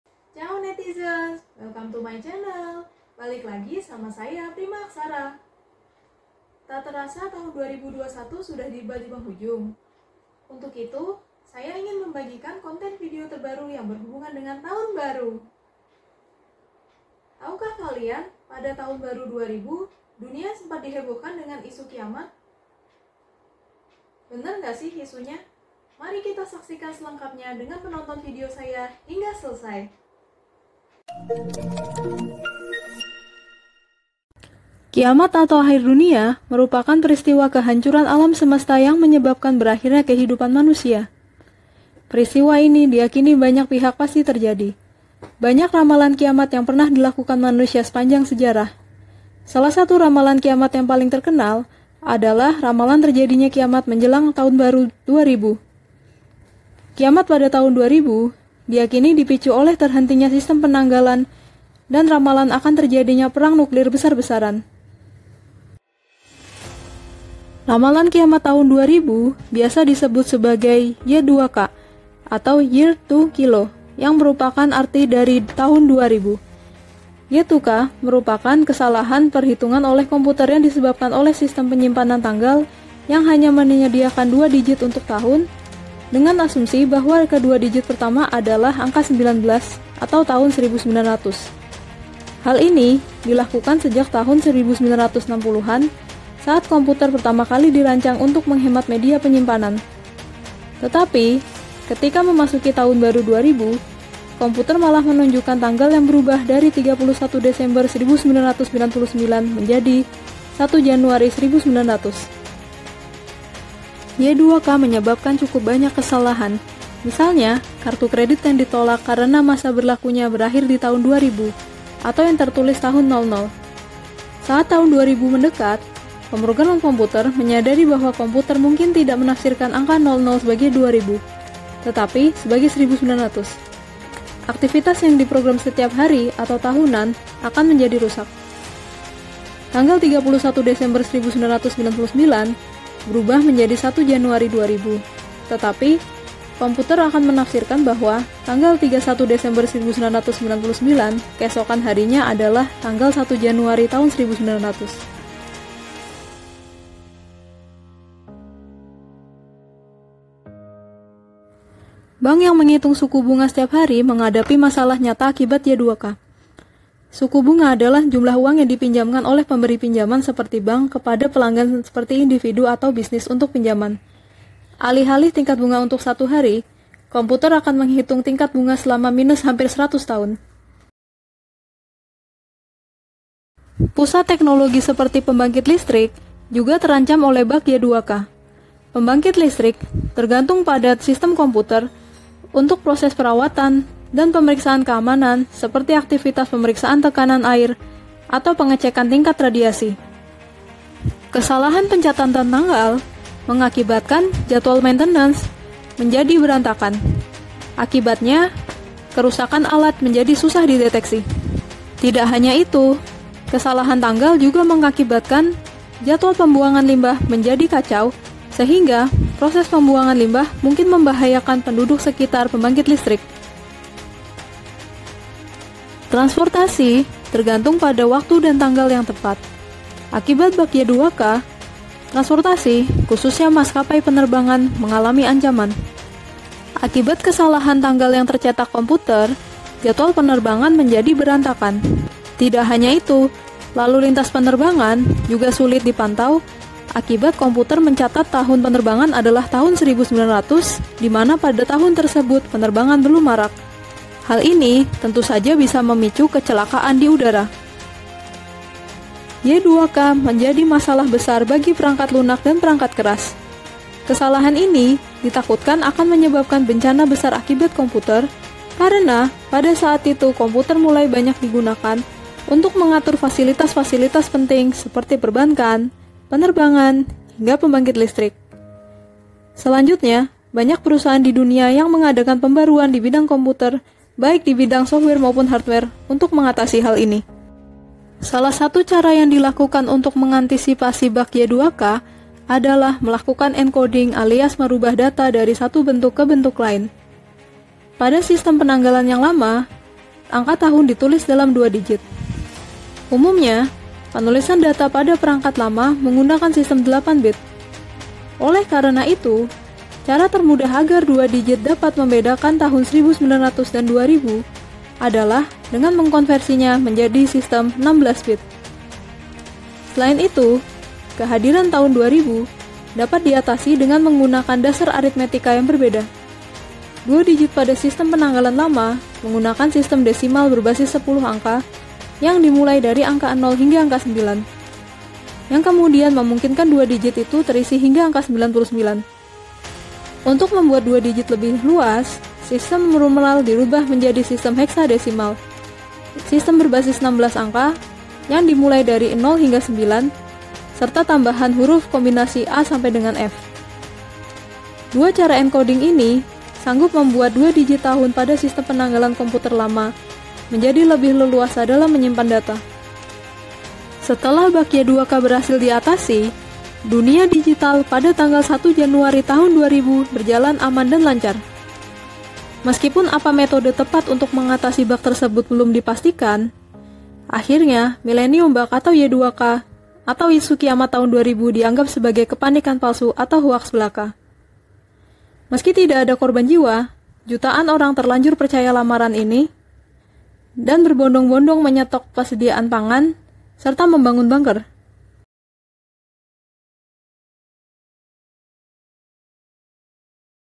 Tahun 2000 KIAMAT!
Tahun 2000 kiamat adalah sebuah peristiwa sejarah yang unik. Kiamat merupakan proses kehancuran alam semesta beserta isinya yang mengancam keberlangsungan hidup manusia. Banyak ramalan kiamat yang pernah terjadi sepanjang sejarah. Salah satu ramalan yang paling terkenal adalah tahun 2000 kiamat. Ramalan tahun 2000 kiamat dipicu oleh kerusakan sistem komputer. Tahun 2000 kiamat hampir saja terjadi jika para ahli tidak melakukan misi penyelamatan.